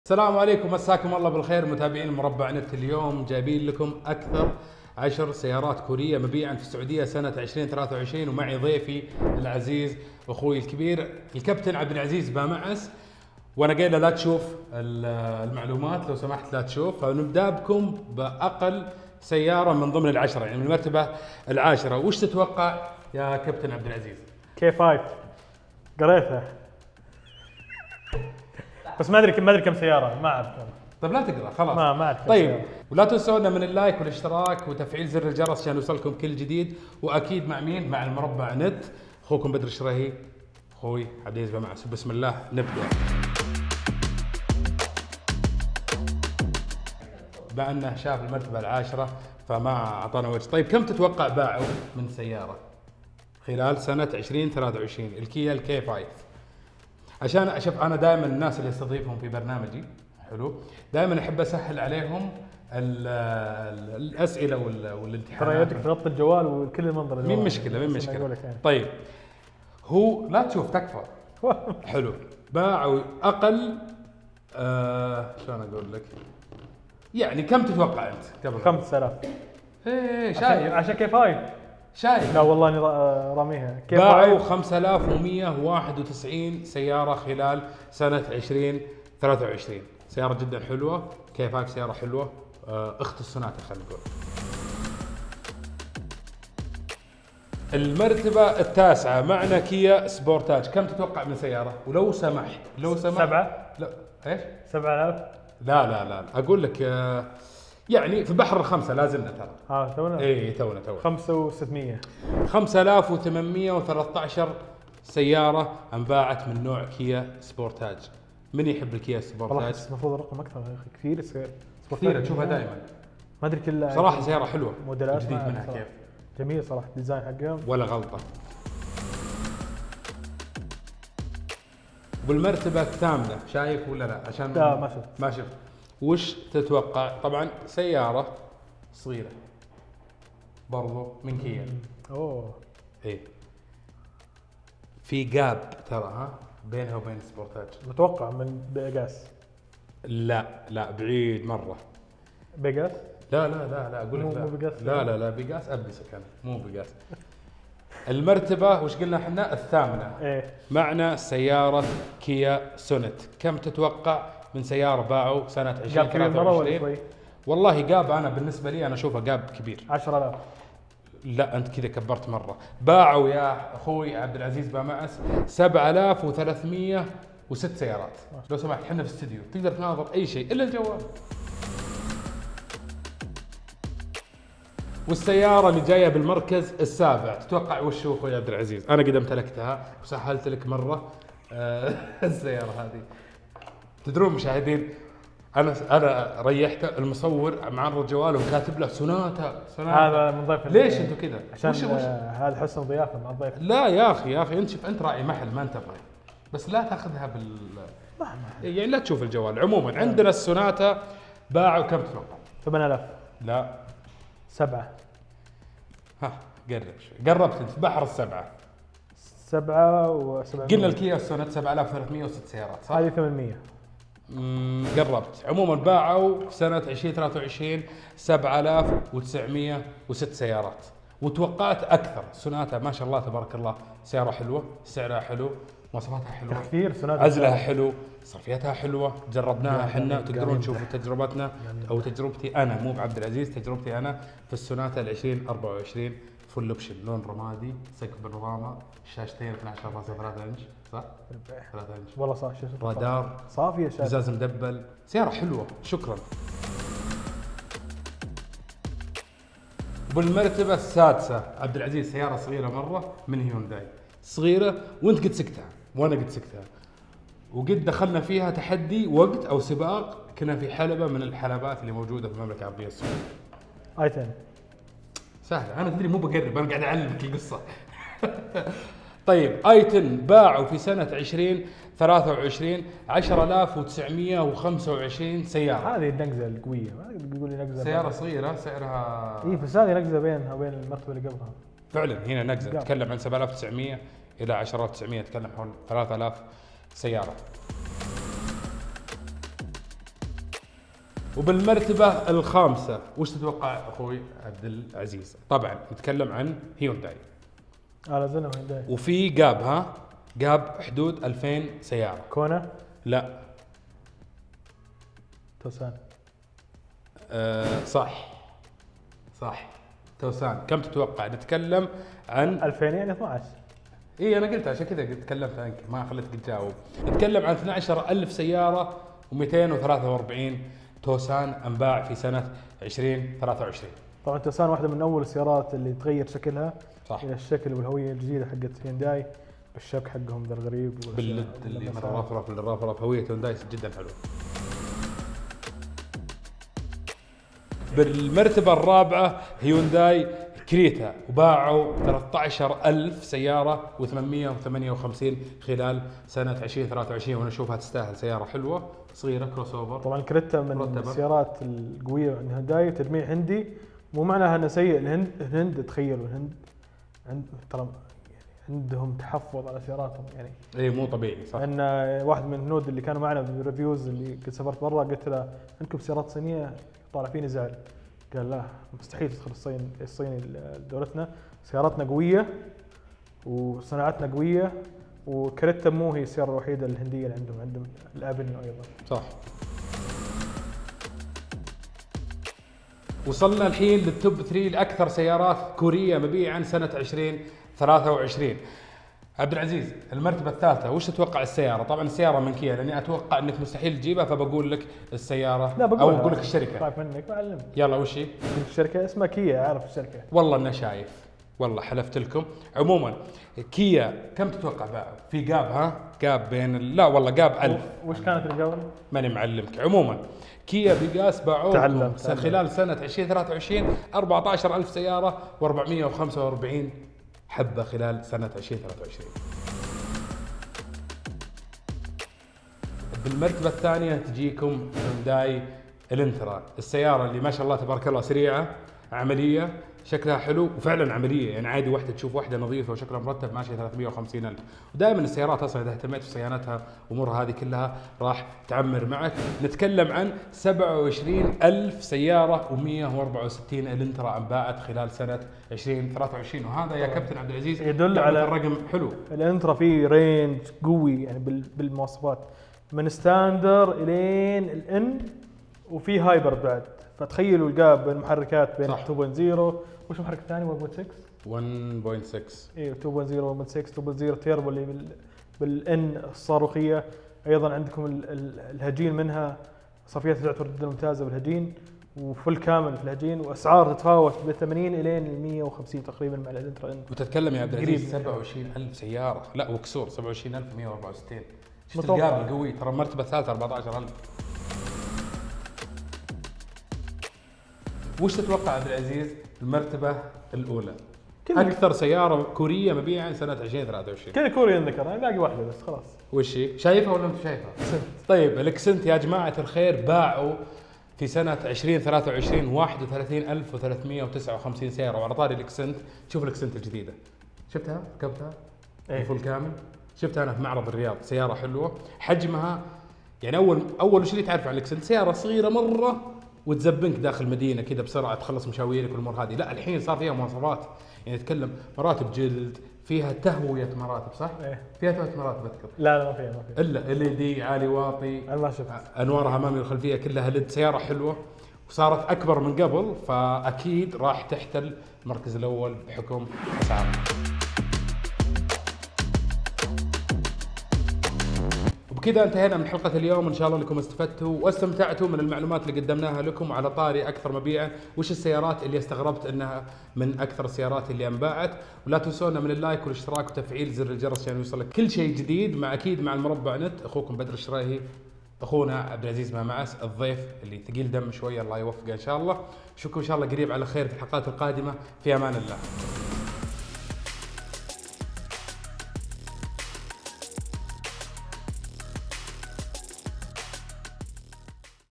توب_تن سيارات كورية تهيمن على سوق السعودية! تعرف على أكثر 10 سيارات كورية مبيعاً في 2023
حمل تطبيق "مستعمل و جديد" وبيع واشتري وتصفح السيارات المستعملة والجديدة المعروضة للبيع او بيع سيارتك